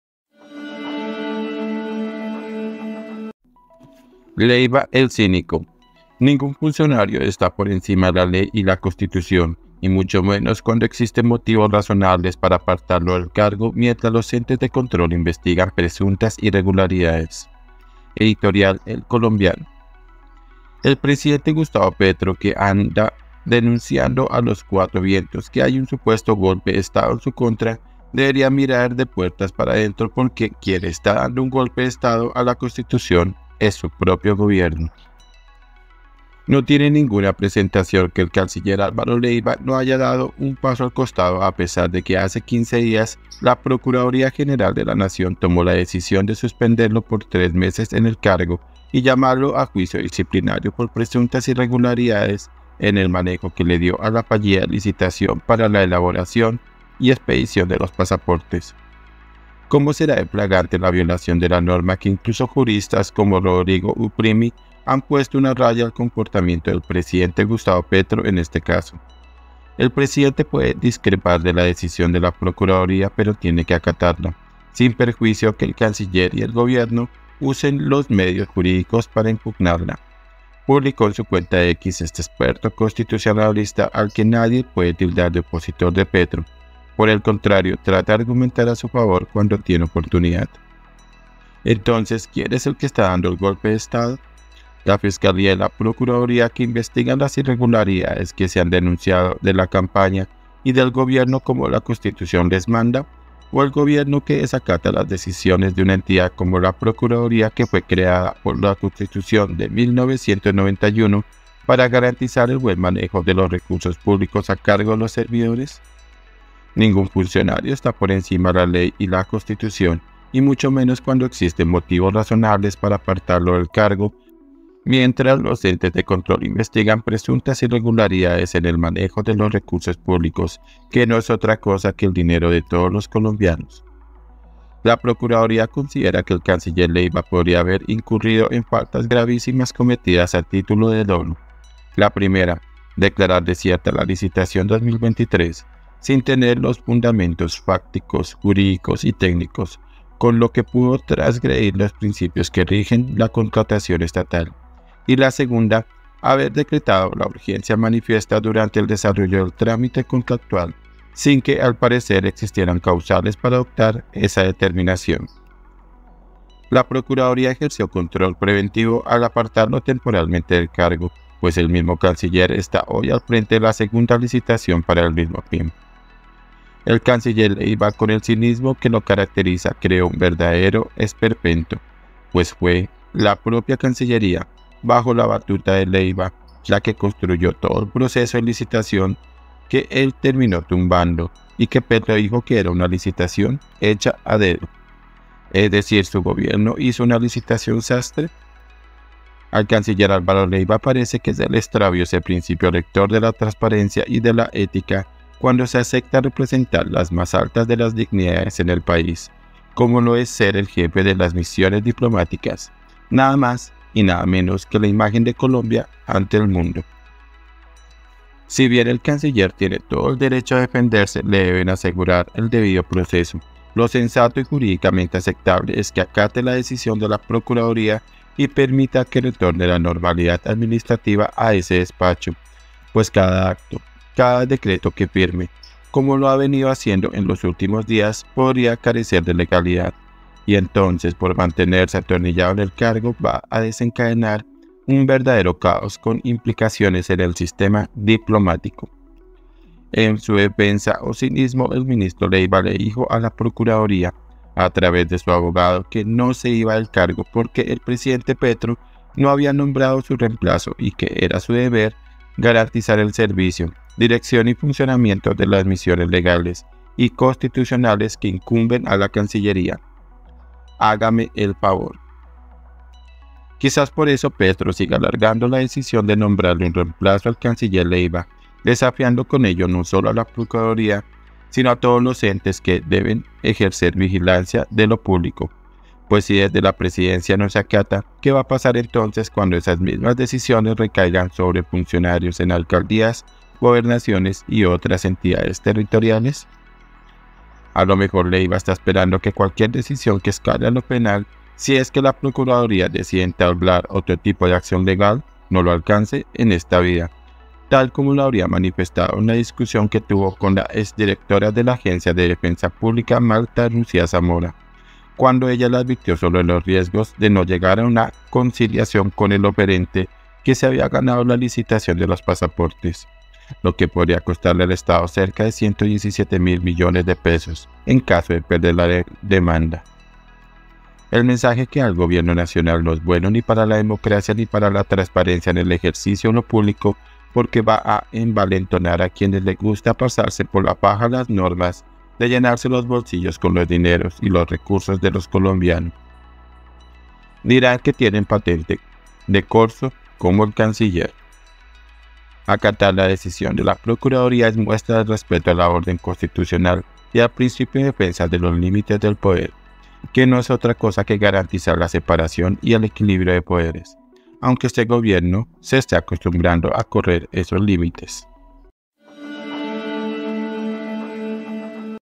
LEYVA EL CÍNICO 🔴 Editorial El Colombiano
Ningún funcionario está por encima de la ley y la Constitución y mucho menos cuando existen motivos razonables para apartarlo del cargo mientras los entes de control investigan presuntas irregularidades.